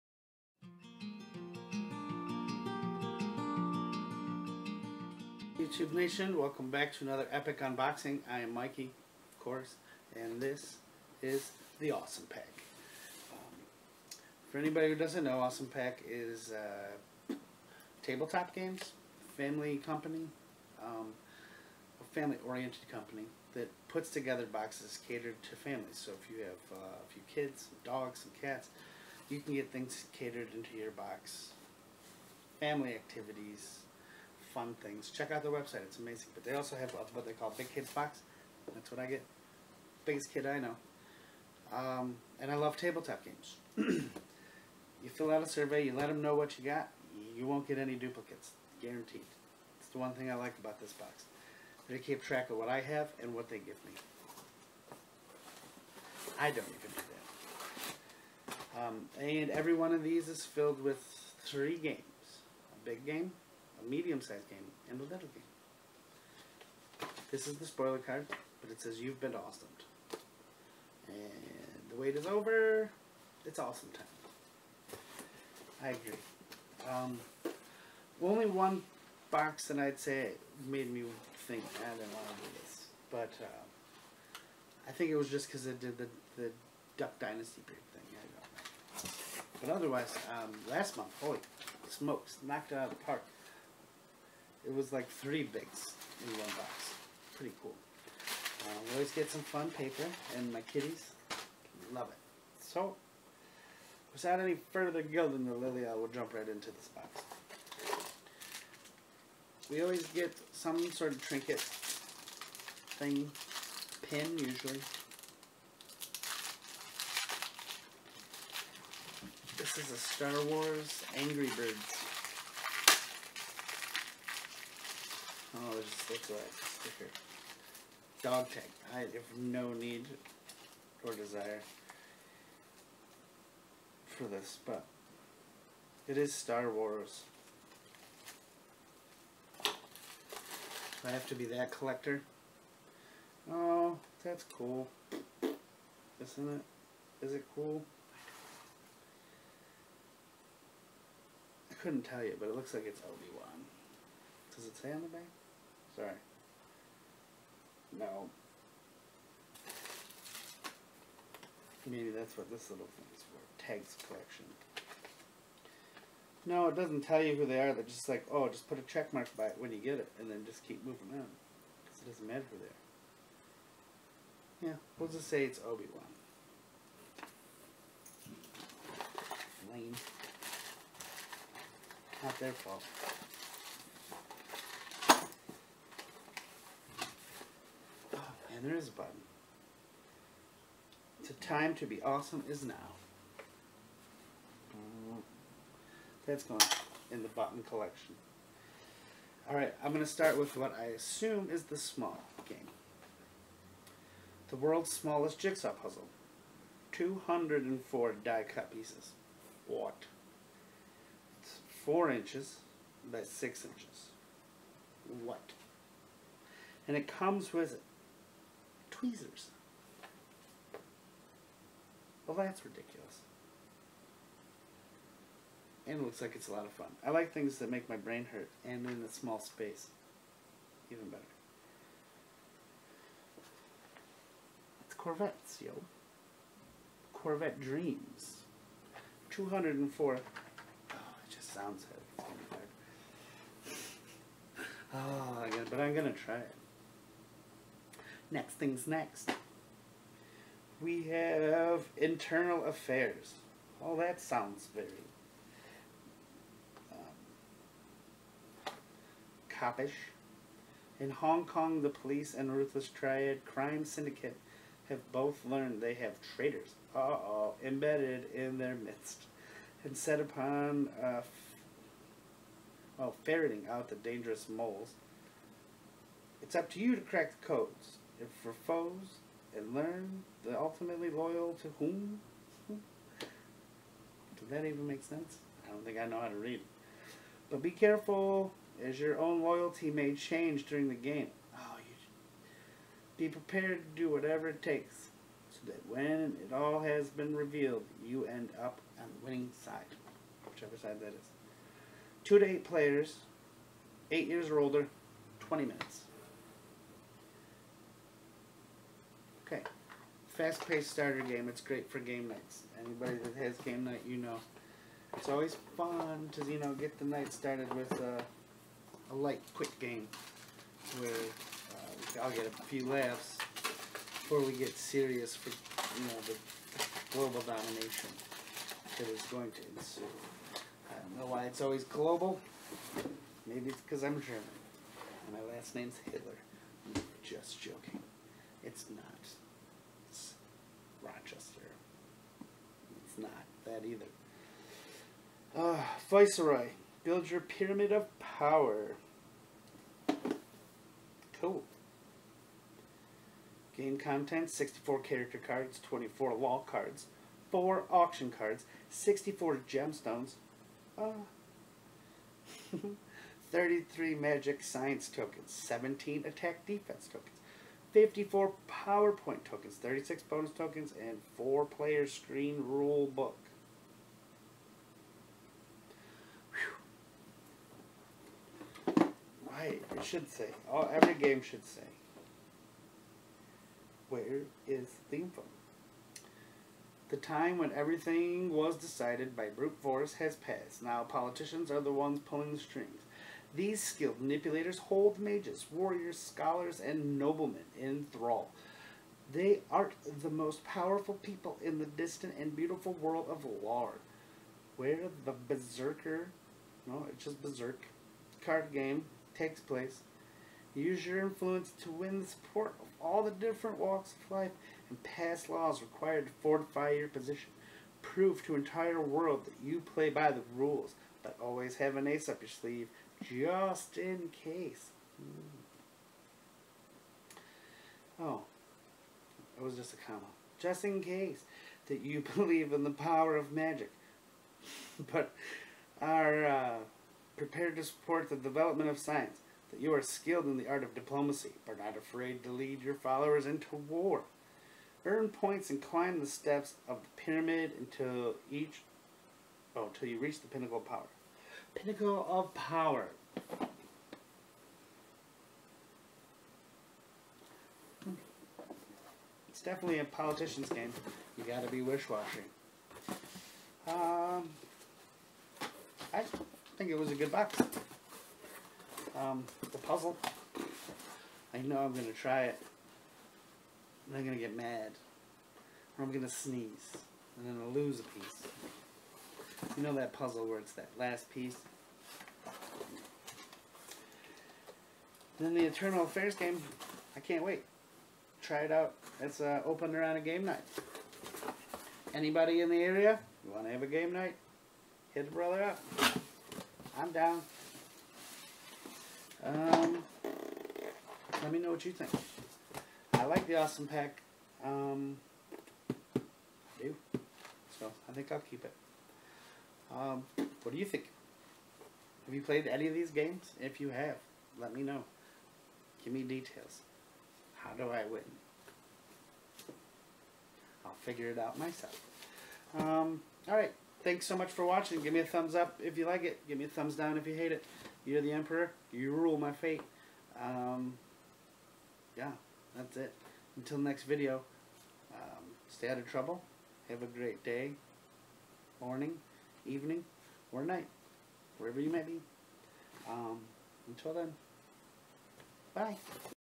July 2017 Awesome Pack
This is a game oriented box, the old school table top kind!